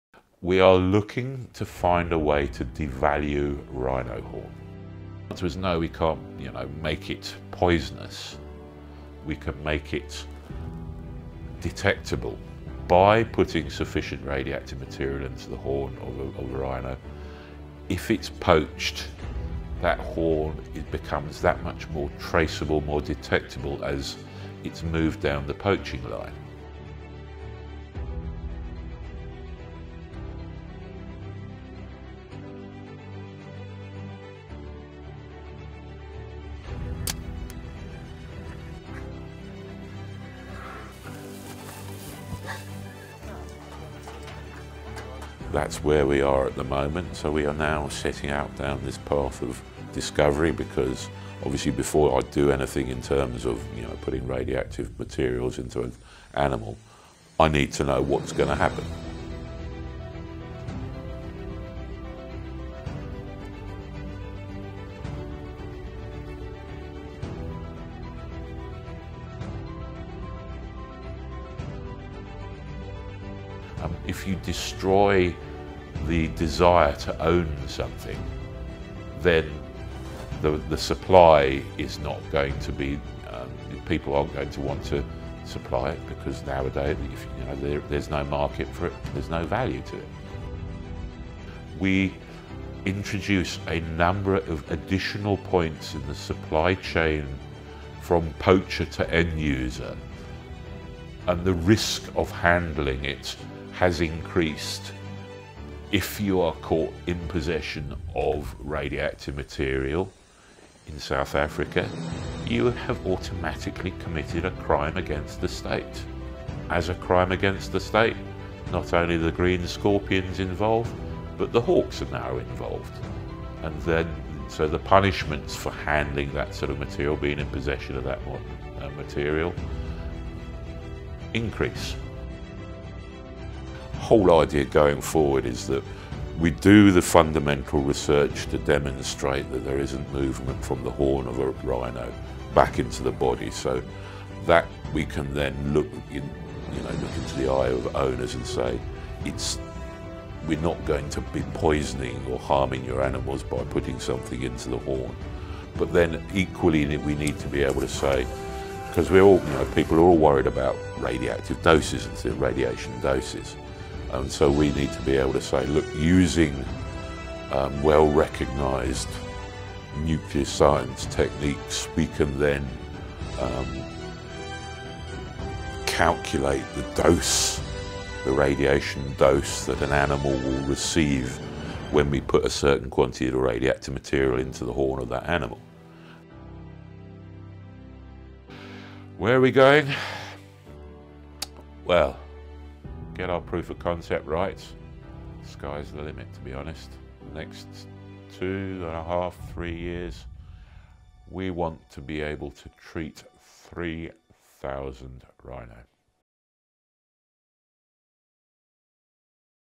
The Rhisotope Project
The Rhisotope Project" was founded two years ago by Prof. James Larkin, the radiation and health physics unit director at Wits University and co-founder  Suzanne Boswell. One a dreamer, the other a scientist. At first, a concept that seemed crazy has received overwhelming support from all corners of the globe, like-minded individuals ready and eager to make a change in the world. Using science-based solutions, we hope to curb the demand for rhino horns and ultimately save a species.

Video filmed and edited by Tracks multimedia Janine Joubert

Click on the 🔗 link 🔗 below to see how you can help us, help them. We cannot do this alone, but with your help! We can.